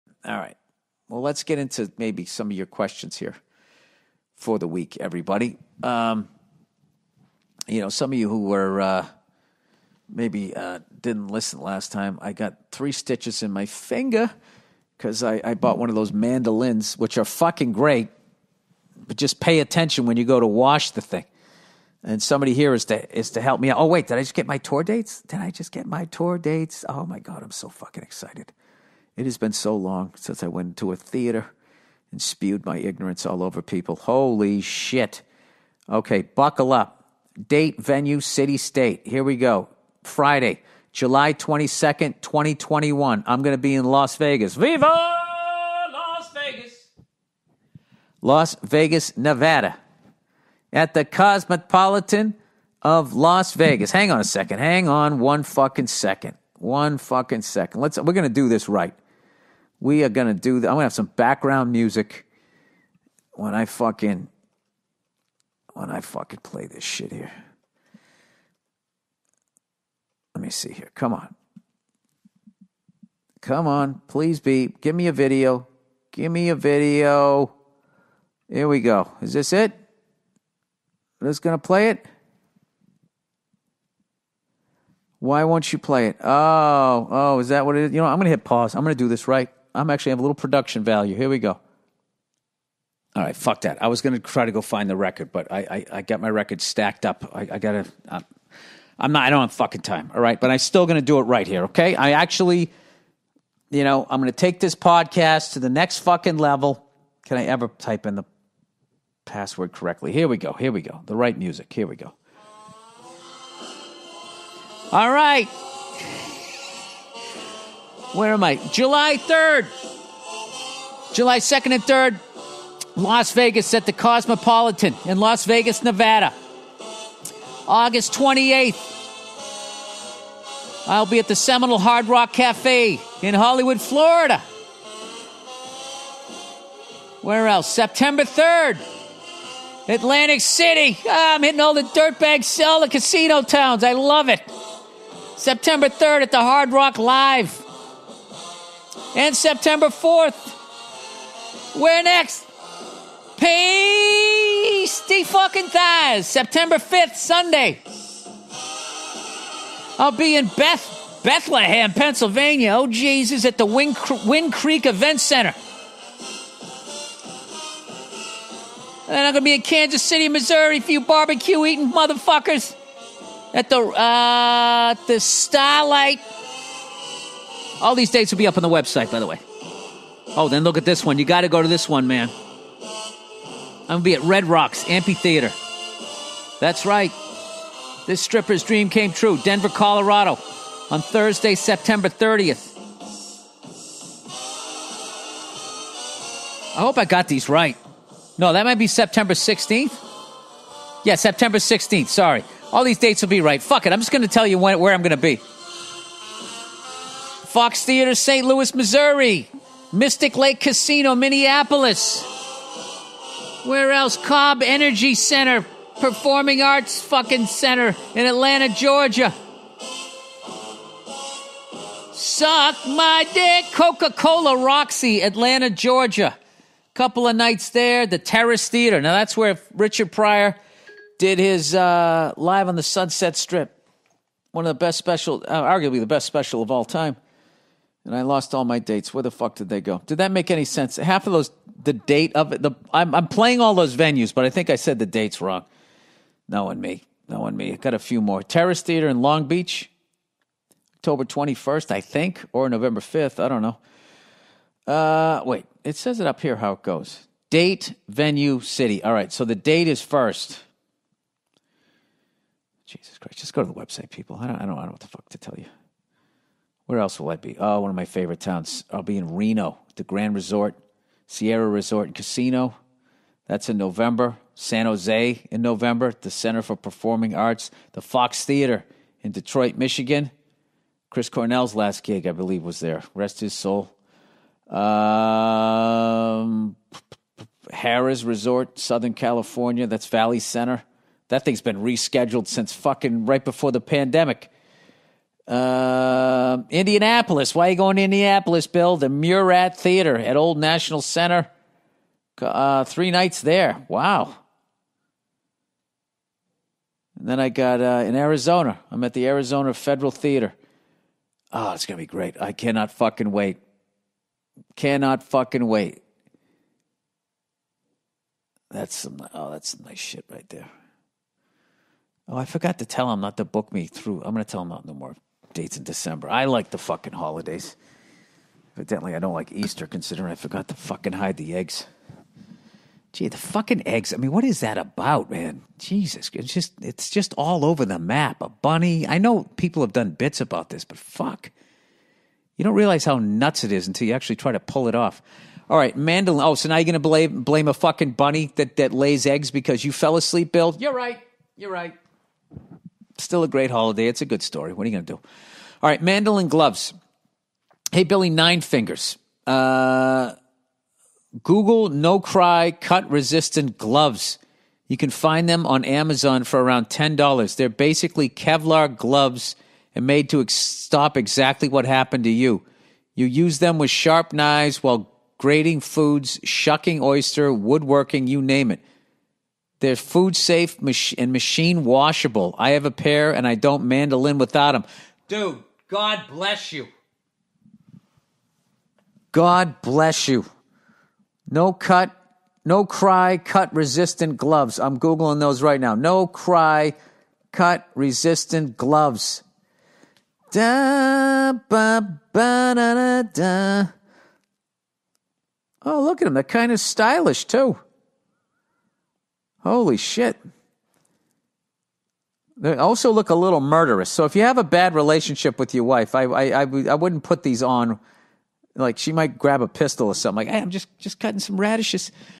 Tour Dates and Mandolin Gloves - Bill Burr
Bill Burr talks about the upcoming tour dates and mandolin gloves.